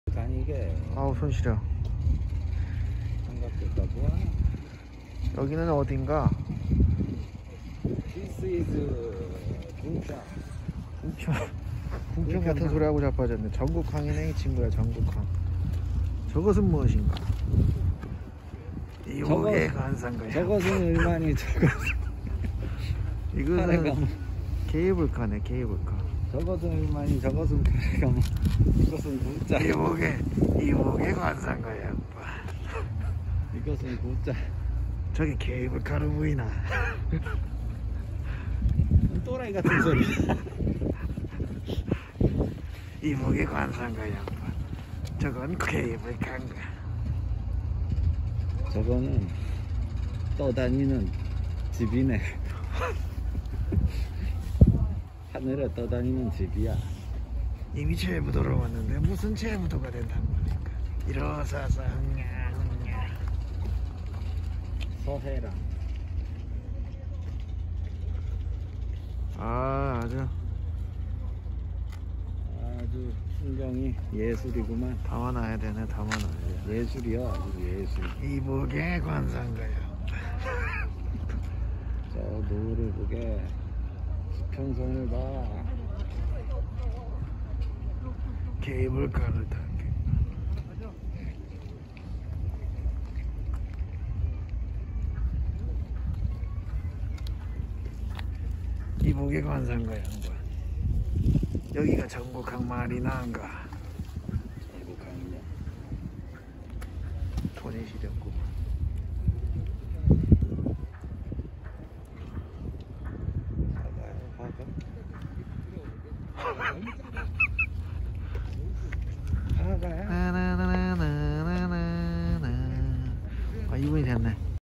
서해에도 에머랄드 바다와 케이블카가 있다면?(전곡항 제부도 케이블카)
1. 숨겨진 여행 명소를 찾아서 소개하고 싶다.
2. 괜찮은 곳이 있으면 영상에 담아본다.
3. 화성에 위치한 전곡항 케이블카 장면